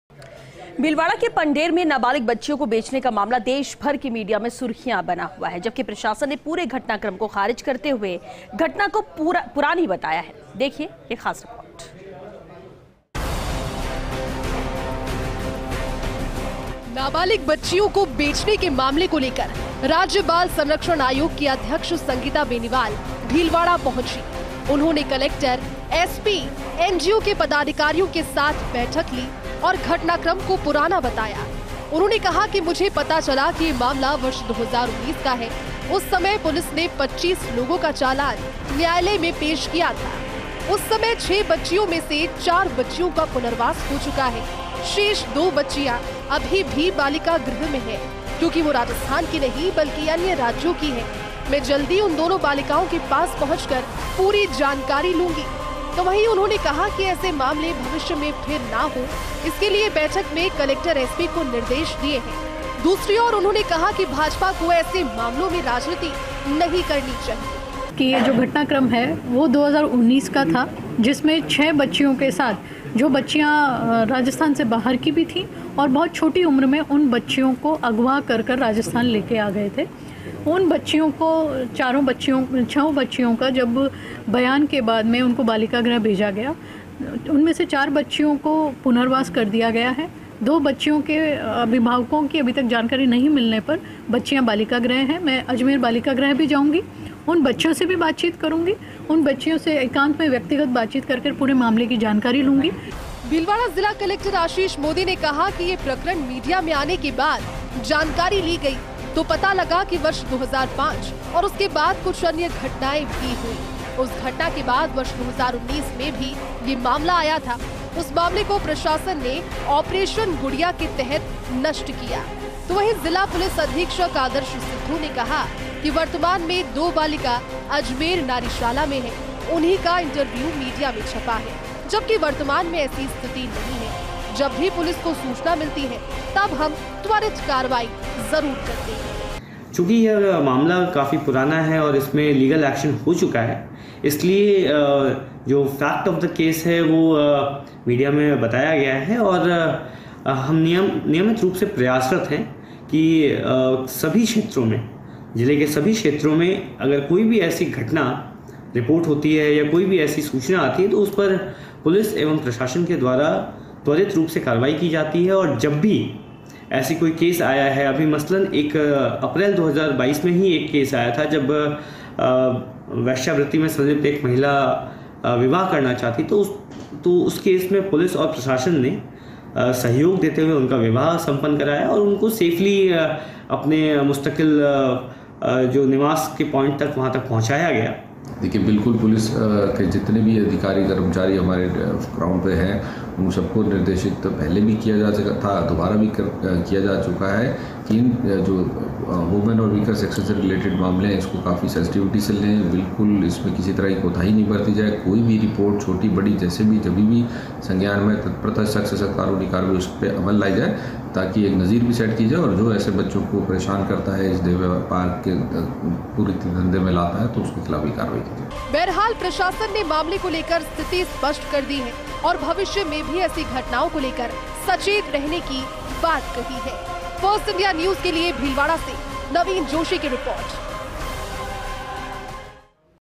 नाबालिग बच्चियों को बेचने का मामला, प्रशासन ने घटनाक्रम को किया खारिज | Bhilwara News
नाबालिग बच्चियों को बेचने का मामला, मीडिया में सुर्खियां बना मामला, प्रशासन ने घटनाक्रम को किया खारिज, पूरे घटनाक्रम को बताया पुराना, राज्य बाल संरक्षण आयोग अध्यक्ष ने ली बैठक, कलेक्टर-SP, NGO पदाधिकारियों के साथ बैठक, संगीता बेनीवाल ने कहा- मामला वर्ष 2019 का, पुलिस ने 25 लोगों का चालान पेश किया था कोर्ट में, भीलवाड़ा | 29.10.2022